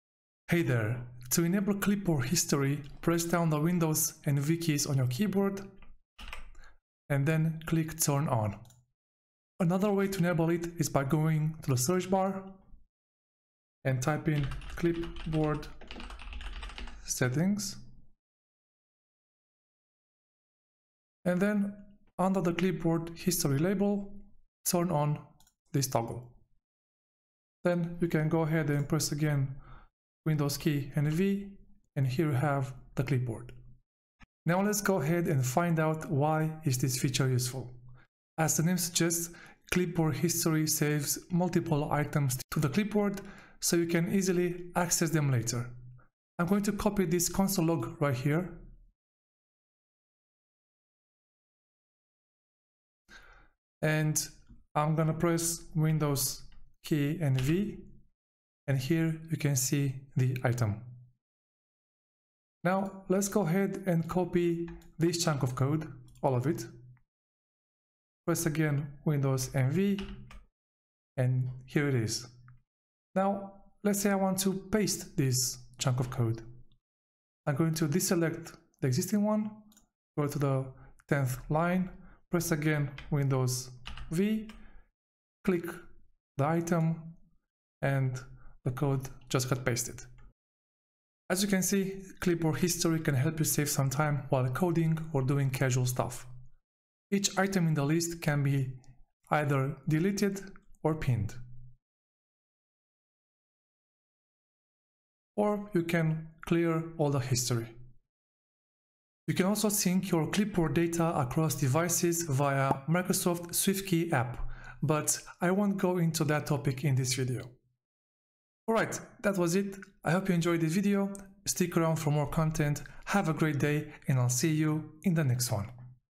How to Enable and Use Clipboard History on Windows 10
Clipboard History is a nice feature that saves multiple items to the clipboard so you can easily access them later.

It could save you some time when coding or doing casual stuff.